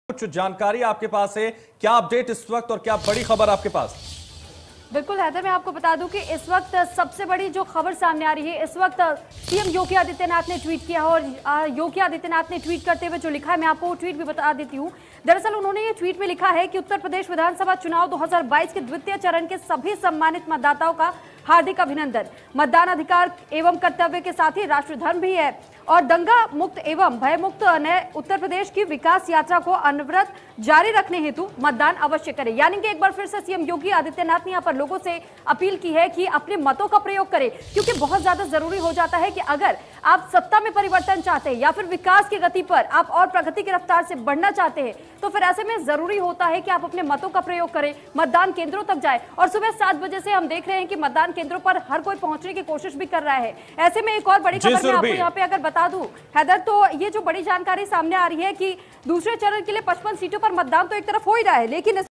दूसरे चरण के मतदान को लेकर CM योगी ने किया ट्वीट
दूसरे चरण के मतदान को लेकर CM योगी ने किया ट्वीट | India News UPUK